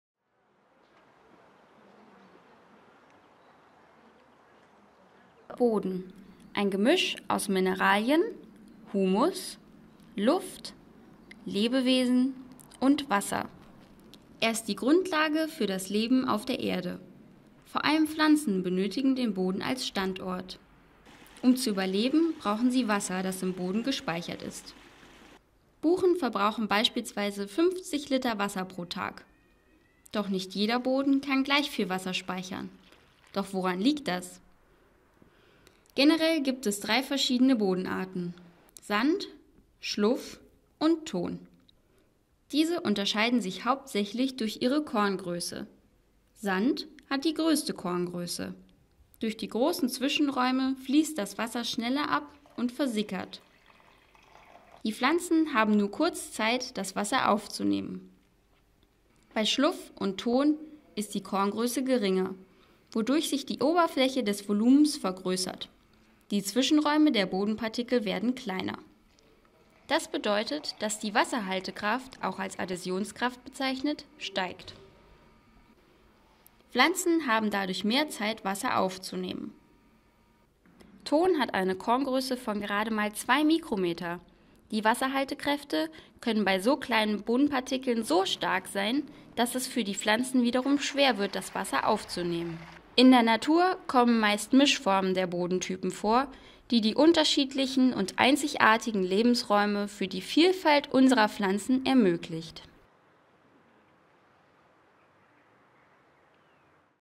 Die Kraft des Bodens
Sand, Schluff und Ton - Drei Bodenarten und ihre unterschiedlichen Eigenschaften. Dieser Clip erklärt's.
Ein Lehrfilm aus dem Kurs "Produktion von Lehrfilmen und Infoclips" der Zentralen Einrichtung für Sprachen und Schlüsselqualifikationen der Uni Göttingen (November 2018)